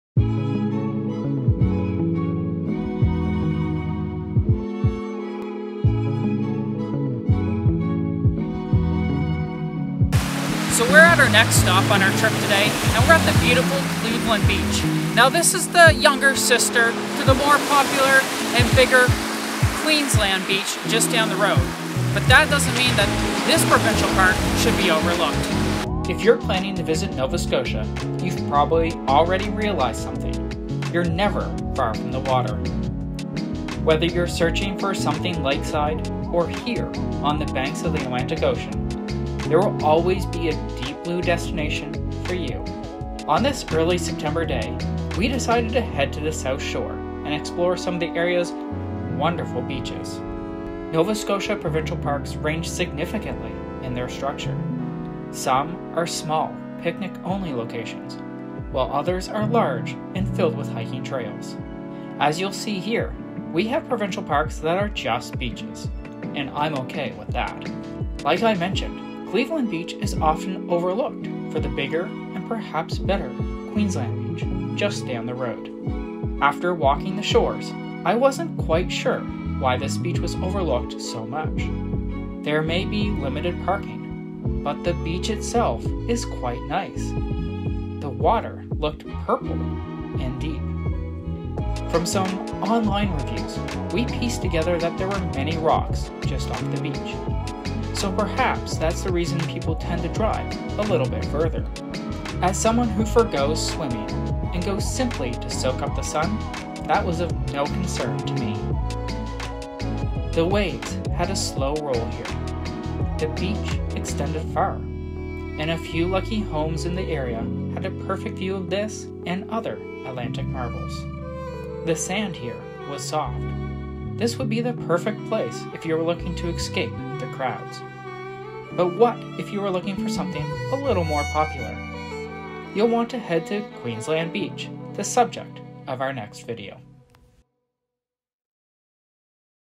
Cleveland Beach Provincial Park | Nova Scotia | Canada
There are fewer things more Nova Scotian than being at the water. A little video about Cleveland Beach Provincial Park, Nova Scotia, Canada